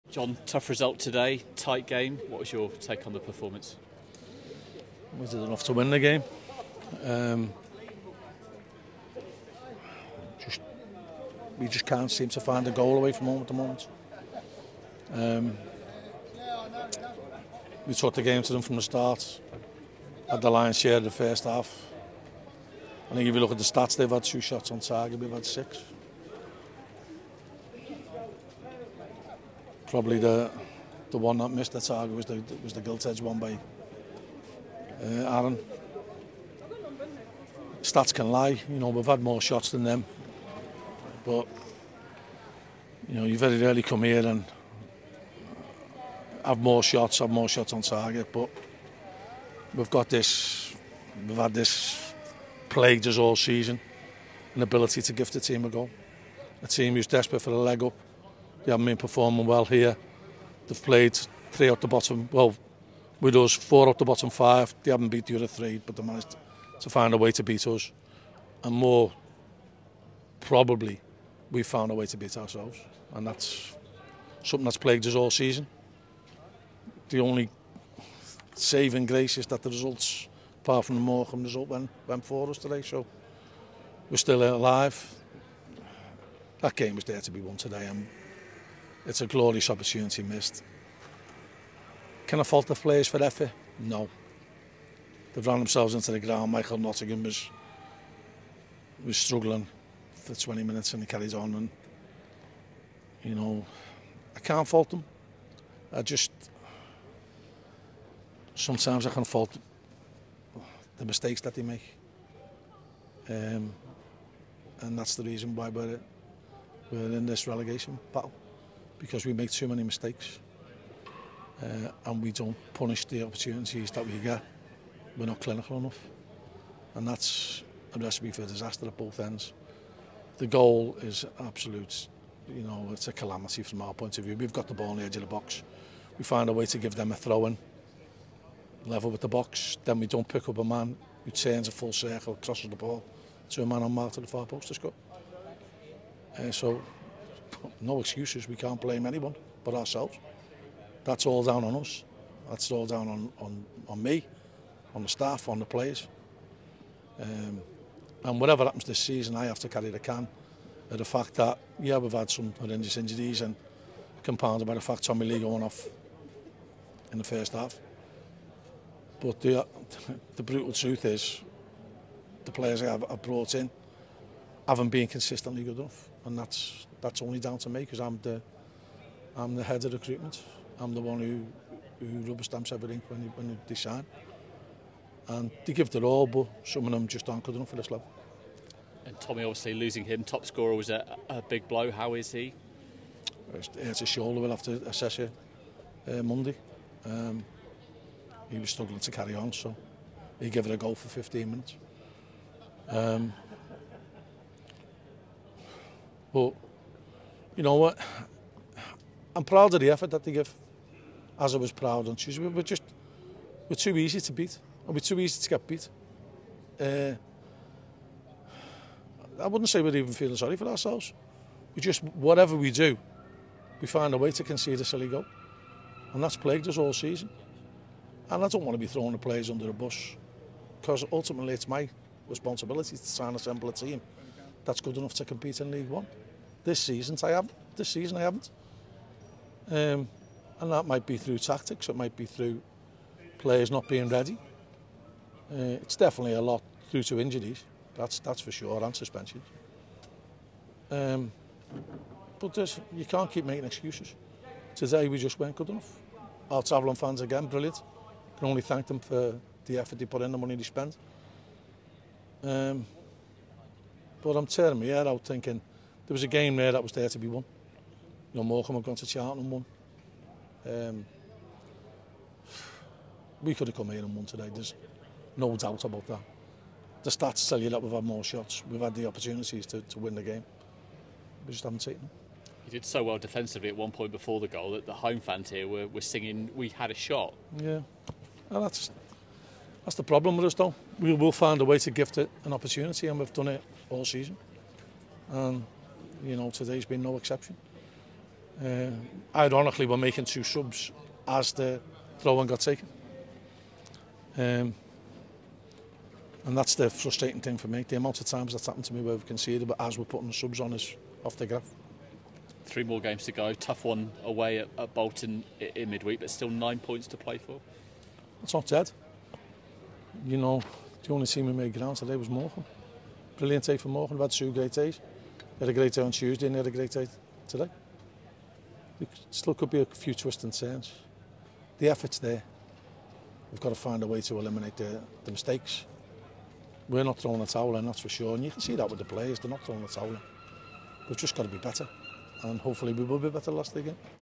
INTERVIEW: Coley's reaction on the defeat to Portsmouth
Accrington Stanley manager John Coleman discusses the 1-0 defeat to Portsmouth.